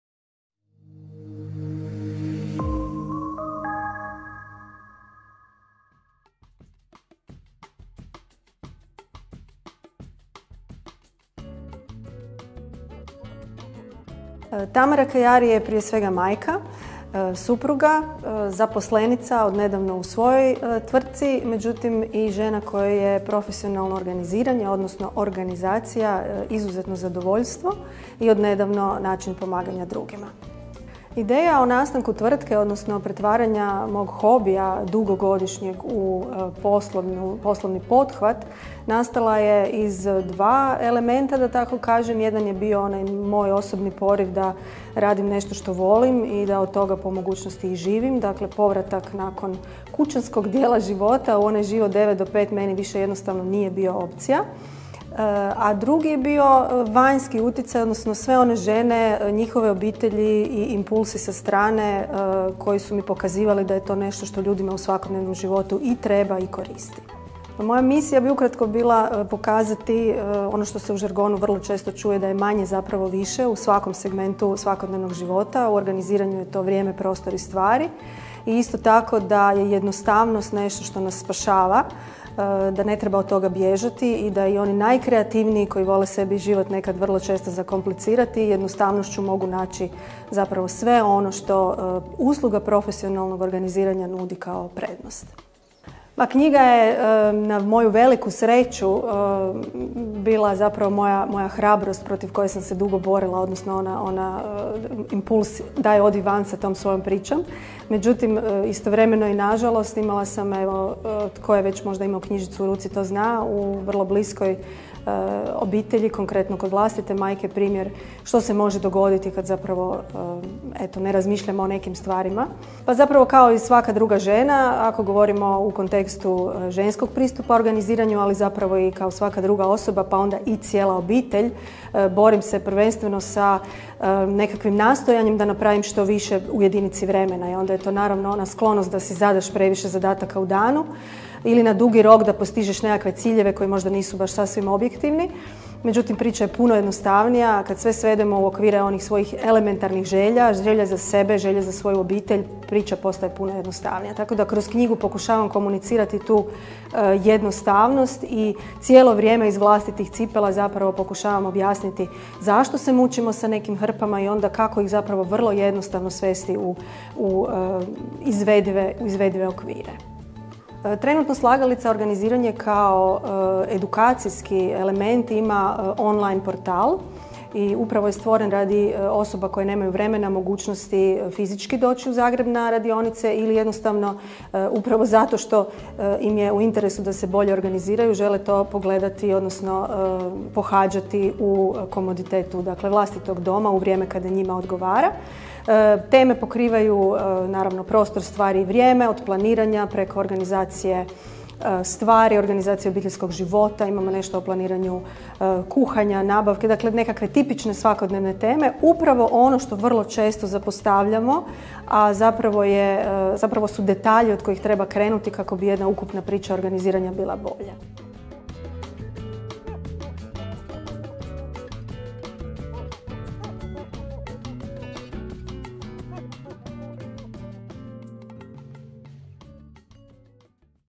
Mame za mame predstavljaju seminar "Organizirana mama"
U sklopu radionica Mame za Mame organizirali smo govanje Tamare Kajari, autorice knjige "Ne daj hrpi da te iscrpi", certificirane profesionalne organizatorice, osnivačice i vlasnice tvrtke "SLAGALICA organiziranje".